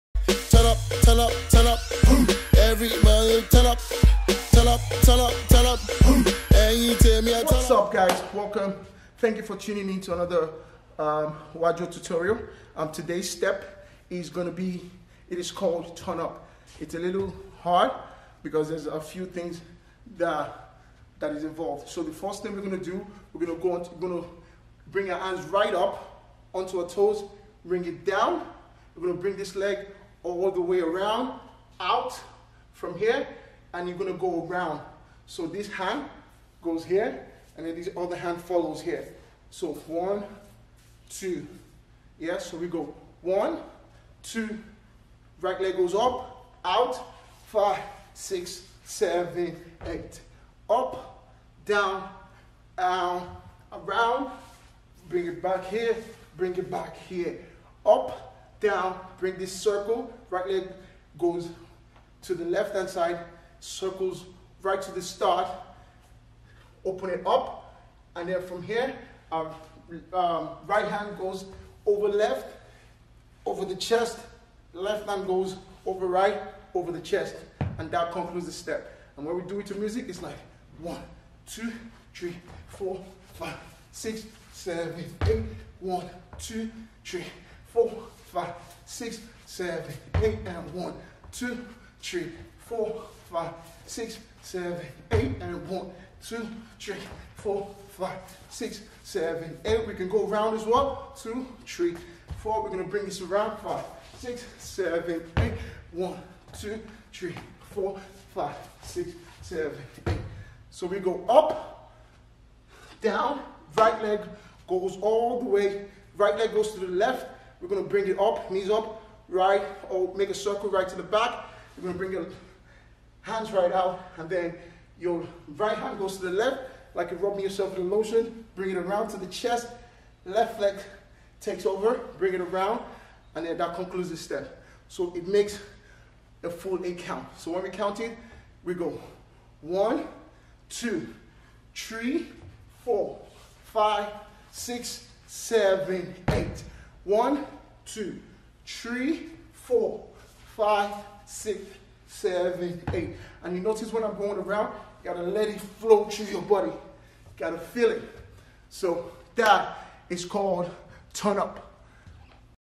TURN UP DANCE TUTORIAL wajoo dance step
WAJOO AFRO DANCE FIT
  BY BOOGIE BEAST

WAJOO dance fitness  is an energetic and high intensity African inspire dance fitness. Its designed to burn fat get you fit movement/grooving and have fun doing it.
Dance your soul free